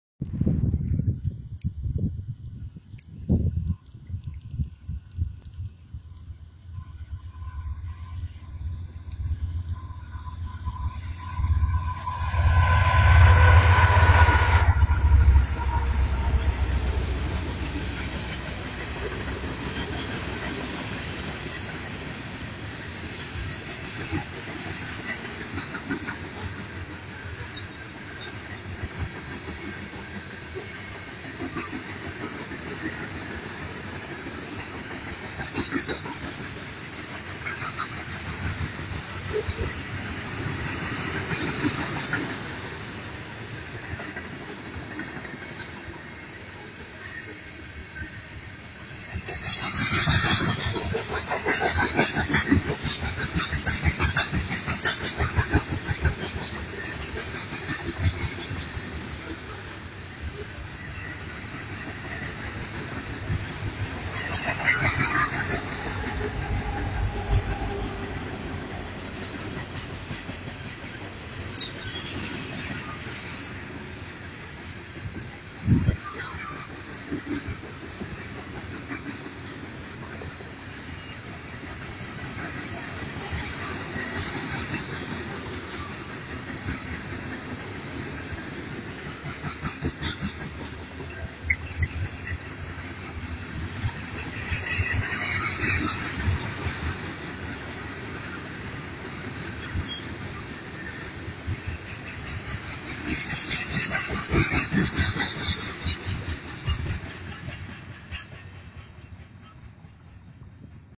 Ferromex 4526, 4022 y Remota 4000, AC4400CW y SD70ACe en León, Guanajuato. México NOAS_5
Aquí les dejo un tren de Ferromex con la locomotora 4526 una AC4400CW de lider y en multiple la 4022 una SD70ACe y Remota la 4000 otra SD70ACe el metalero cargado al norte pasando la placa kilométrica # 409 de la línea A del distrito de León, en León, Guanajuato. México NOAS_5